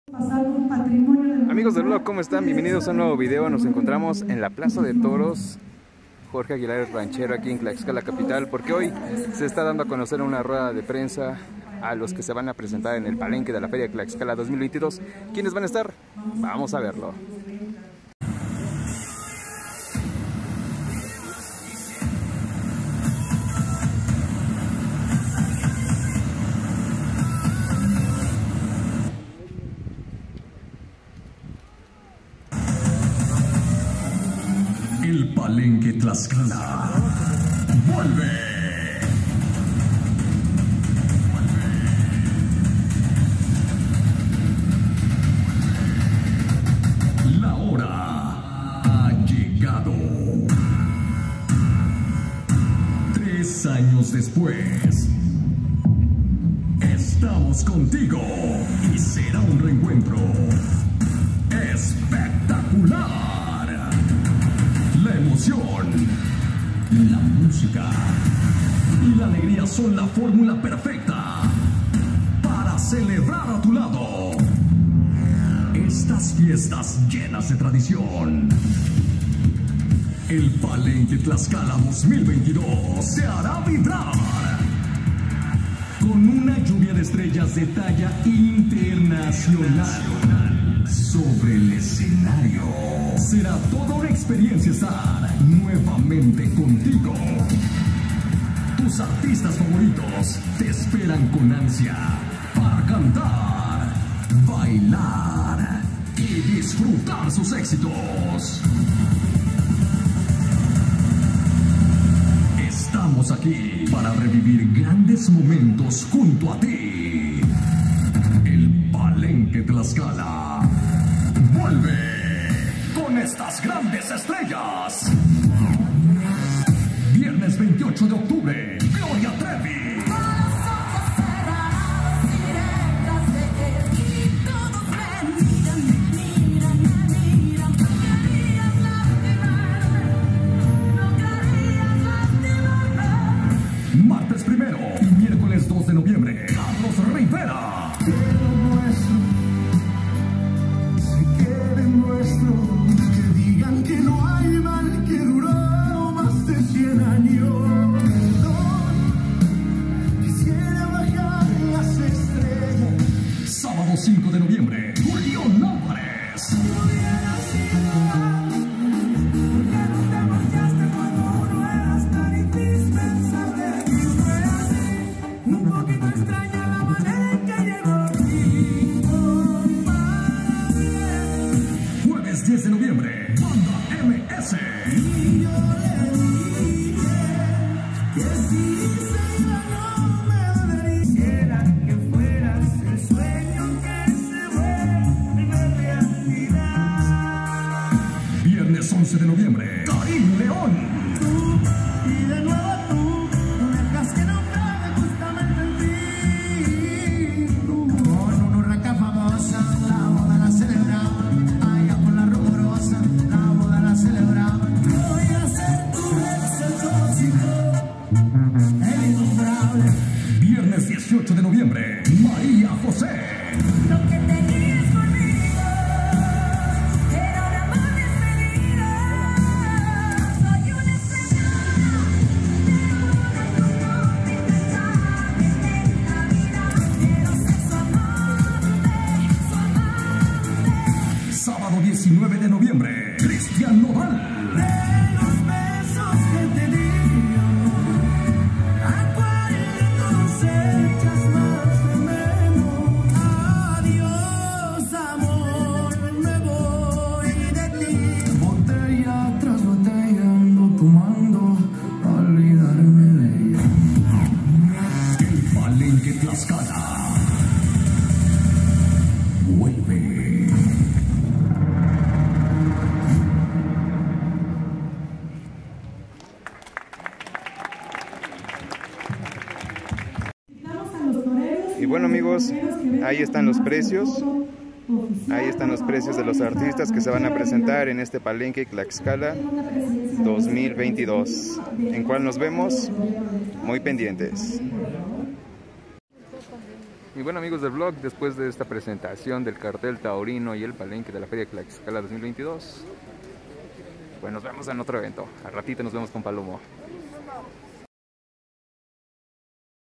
ELLOS estarán en el PALENQUE de la FERIA TLAXCALA 2022
Se presentó el cartel del palenque de la Feria Tlaxcala 2022 y ellos estarán presentes.